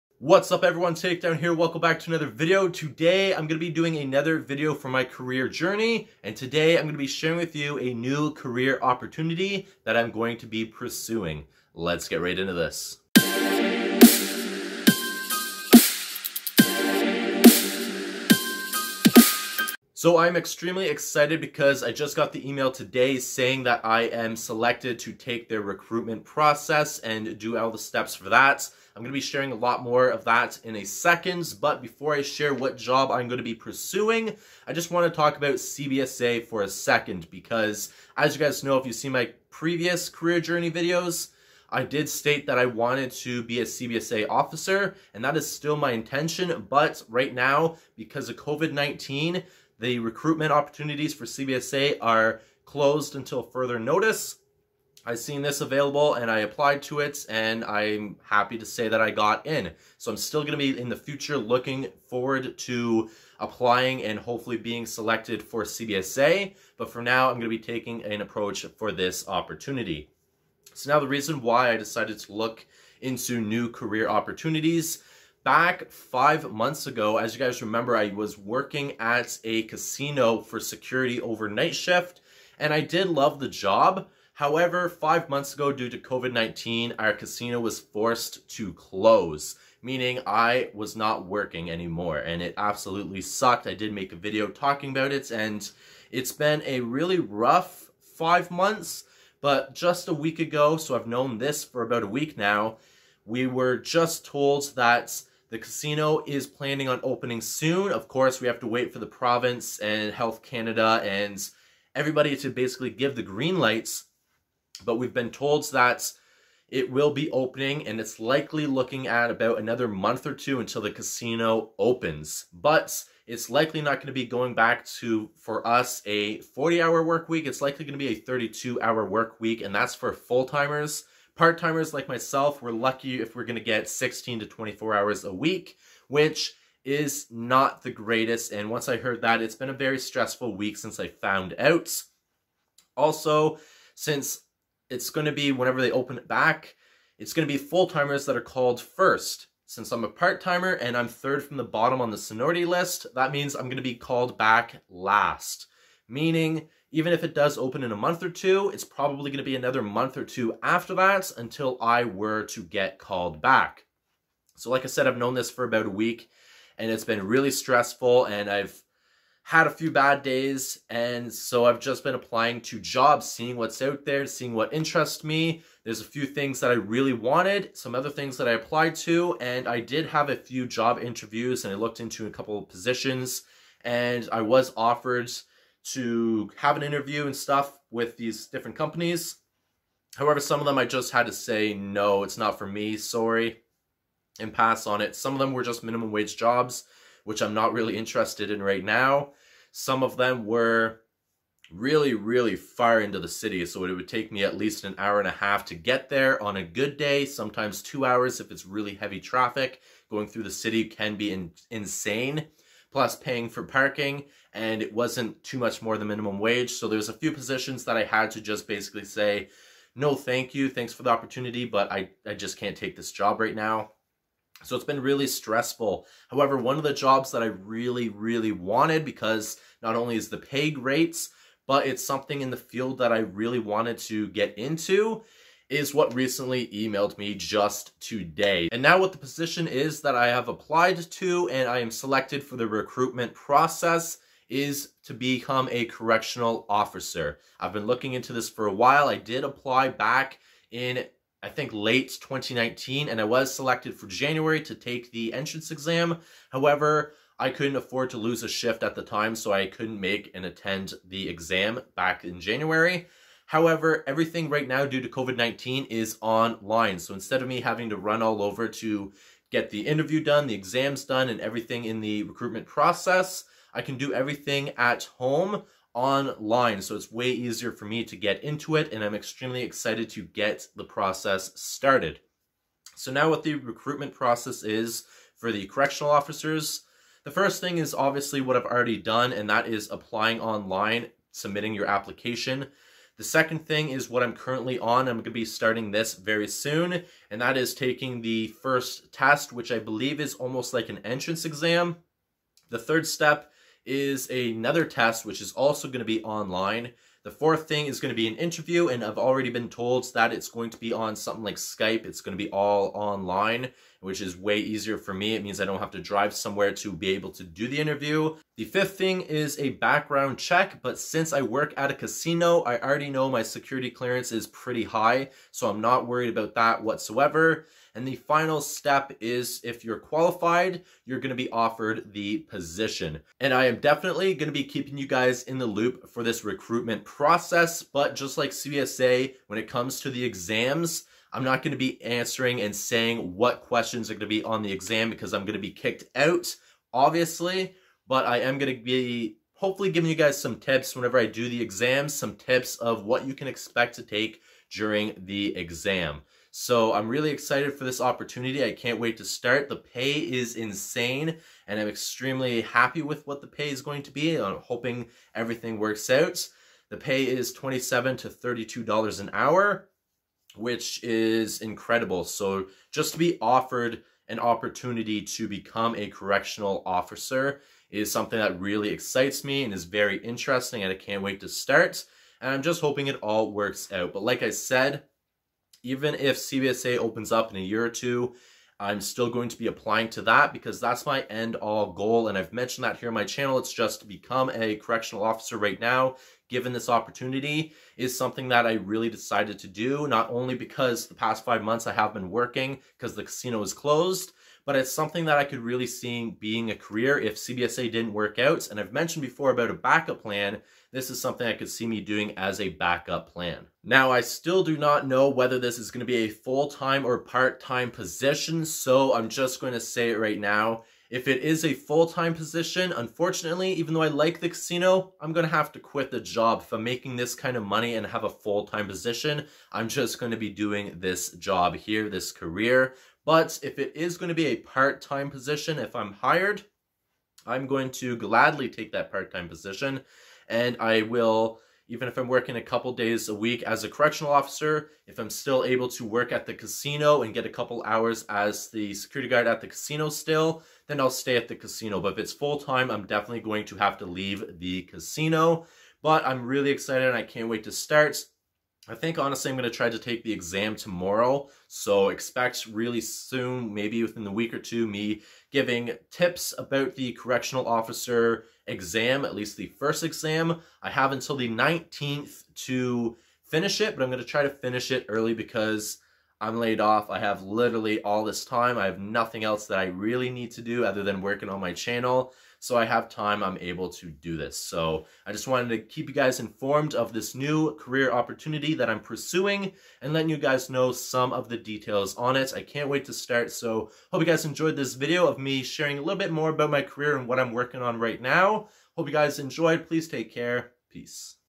NEW CAREER OPPORTUNITY || CORRECTIONAL OFFICER
A new career opportunity is upon me! I have been selected for the recruitment process to become a correctional officer. I will share this journey with you throughout the process. I am extremely excited for this new chapter!